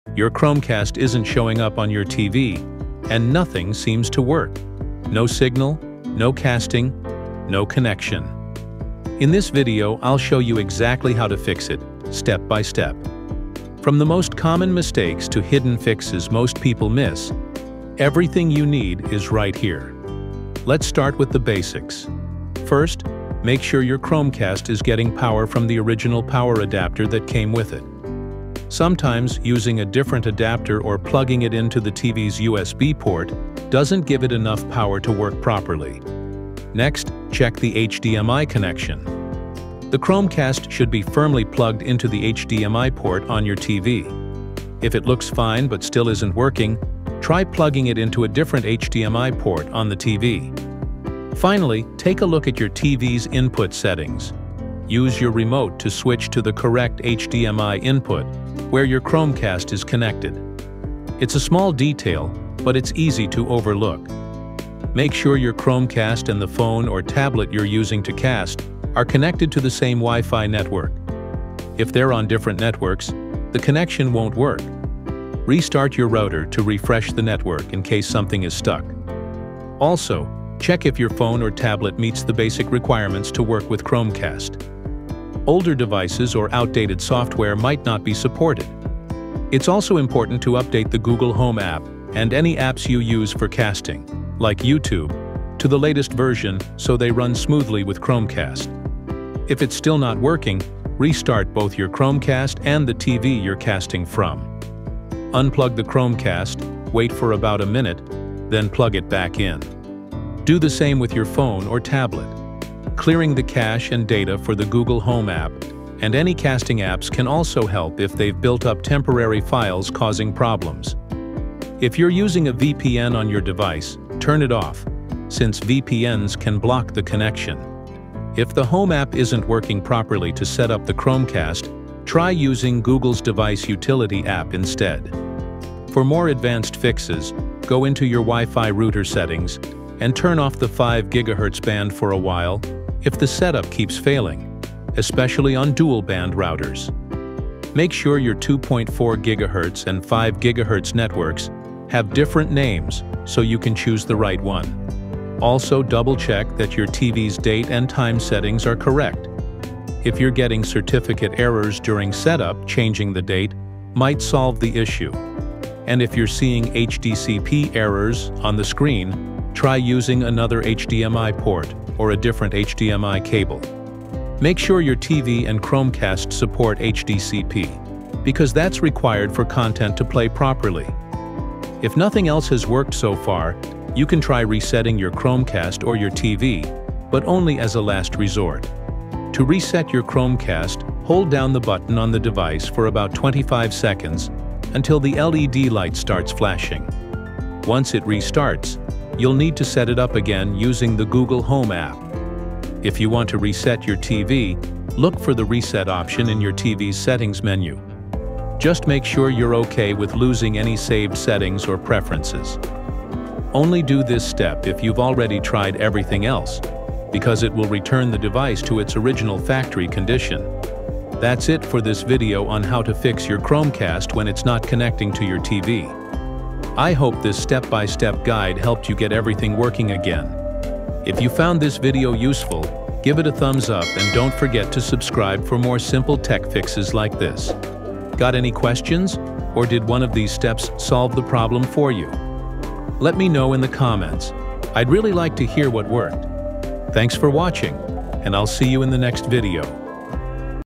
Chromecast Not Connecting to TV – Step-by-Step Fix Guide
Is your Chromecast not connecting to your TV no matter what you try? No signal, no casting, no connection—it’s frustrating, but you’re not alone. In this video, I walk you through step-by-step solutions to fix a Chromecast that won’t show up or connect to your television. Whether it's a power issue, an HDMI mistake, a Wi-Fi mismatch, or something more hidden—this guide has got you covered.

We’ll start with simple checks like using the original power adapter and the correct HDMI input, and then move on to deeper fixes like clearing app cache, checking for VPN issues, and tweaking your router settings. I’ll also explain what to do if your TV gives you HDCP or certificate errors during setup—and when it’s time to try a full reset of your Chromecast or TV.

This video is perfect for beginners or anyone who just wants a clear and easy fix without all the tech jargon. So if your Chromecast is not working or connecting, don’t worry—let’s get it sorted together!